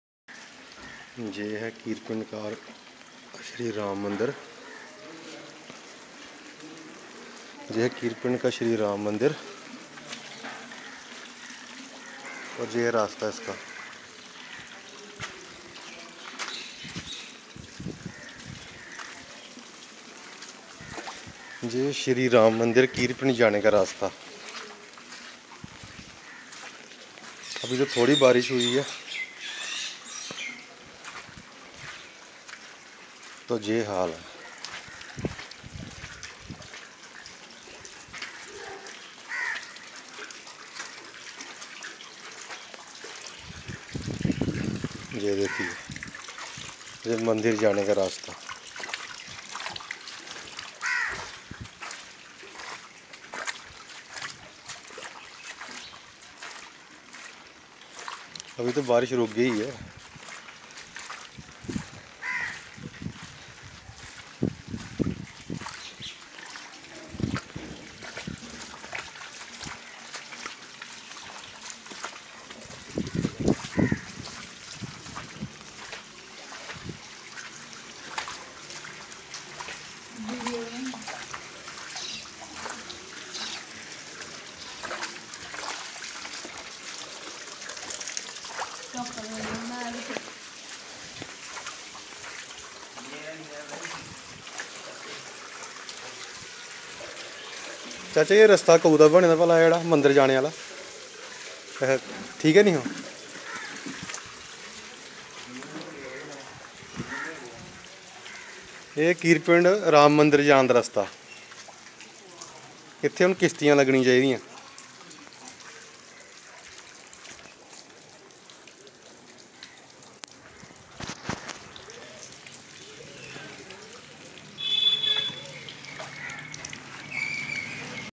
यह दुर्दशा है आरएसपुरा क्षेत्र के गांव किरपिंड़ में स्थित राम मंदिर को जाने वाली गली की...
यह दुर्दशा है आरएसपुरा क्षेत्र के गांव किरपिंड़ में स्थित राम मंदिर को जाने वाली गली की... जिसे देखकर आप अंदाजा लगा सकते हैं कि ग्रामीण स्तर पर कितना विकास हुआ है और हो रहा है.